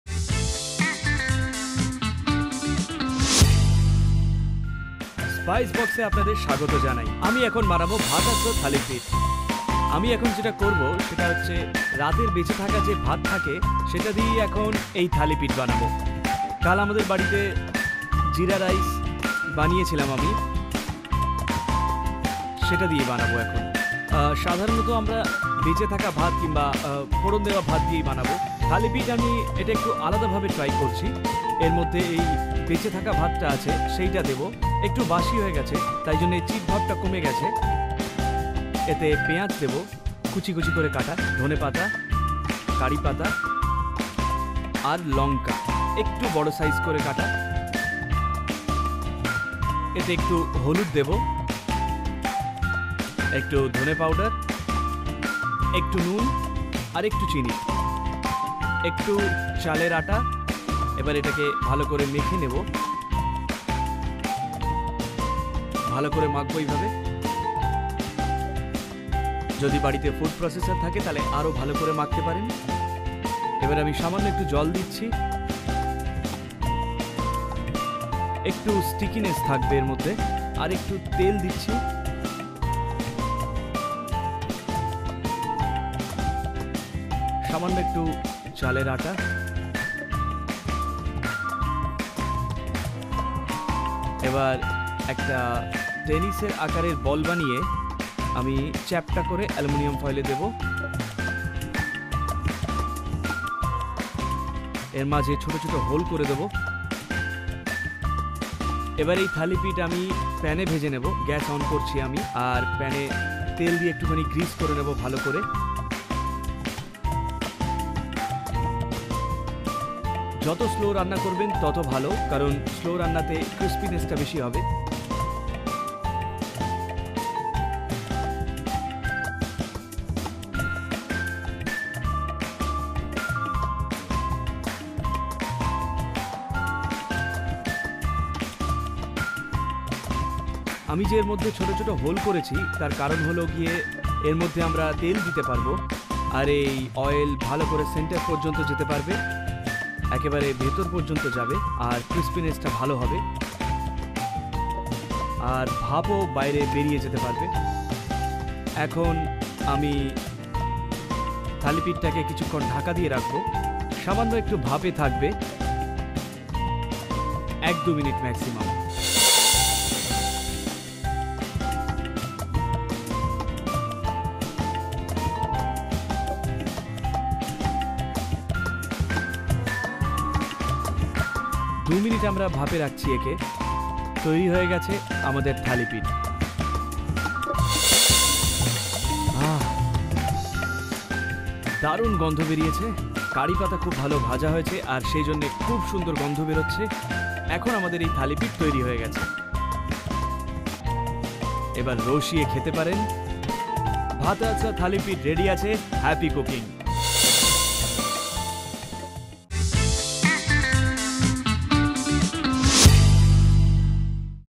ভাটাচে থালিপিঠ (Bhatache thalipeeth)
উপকরণ -

জিরা রাইস ১ বাটি
পিঁয়াজ কুচি ১ কাপ
ধনেপাতা কুচি ২ চামচ
কারিপাতা ৮-১০টি
কাঁচালঙ্কা কুচি ২-৩টি
হলুদ গুঁড়ো ১ চামচ
ধনে গুঁড়ো ১/২ চামচ
চিনি ১ চামচ
চালের গুঁড়ো ২ চামচ
নুন স্বাদমতো
তেল পরিমাণমতো
জল প্রয়োজনমতো

পদ্ধতি -

একটি বাটিতে জিরা রাইস নিন।
তাতে দিন কোচানো পিঁয়াজ, ধনেপাতা কুচি, কারিপাতা, কাঁচালঙ্কা কুচি, হলুদ গুঁড়ো, ধনে গুঁড়ো, নুন, চিনি, চালের গুঁড়ো।
সামান্য জল ও তেল দিয়ে সমস্ত উপকরণগুলো মেখে নিন।
মিশ্রণটি থেকে বলের আকারে গড়ে অ্যালুমিনিয়ামের ফয়েলের উপর রাখুন।
হাতে চাপে থালিপিঠ চ্যাপ্টা করে নিয়ে তাতে গর্ত করে দিন।
গ্যাসে প্যান বসিয়ে সামান্য তেল গরম করুন।
তেল গরম হলে তাতে বানিয়ে রাখা থালিপিঠ দিয়ে দিন।
থালিপিঠের গর্তে অল্প অল্প করে তেল ঢেলে প্যান ঢেকে ভাপিয়ে নিন।
এরপর এপিঠ-ওপিঠ ভেজে নিলেই তৈরি বাটাচে থালিপিঠ।